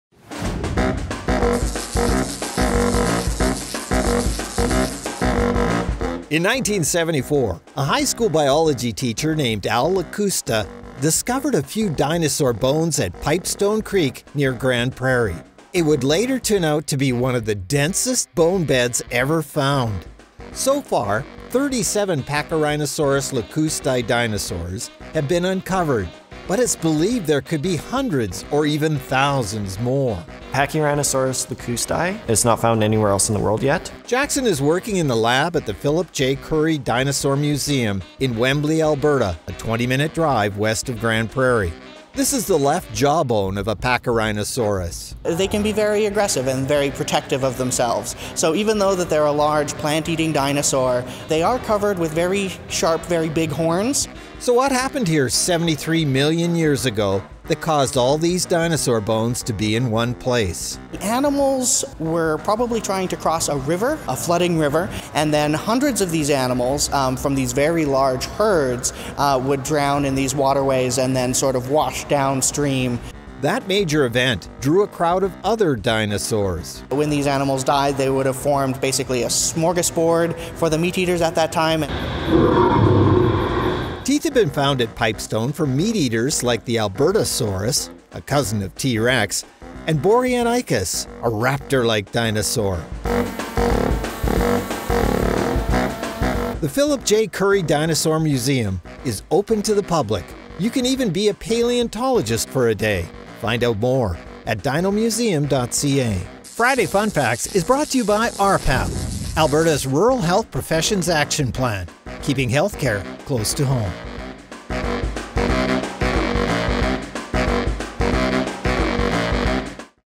Thousands of dinosaurs once lived near Wembley
In the 1970s, a high school biology teacher found dinosaur bones near Wembley, AB, about 20 minutes west of Grande Prairie.

Since then thousands of bones have been uncovered at the Pipestone Creek bonebed leading to the discovery of a new plant eating dinosaur called Pachyrhinosaurus lakustai.

So far, 37 Pachyrhinosaurus dinosaurs have been unearthed but paleotologists believe there could be hundreds or even thousands more.

What happened 73 million years ago, that they all ended up here?

Join us for a trip to the Philip J. Currie Dinosaur Museum in Wembley to find out why.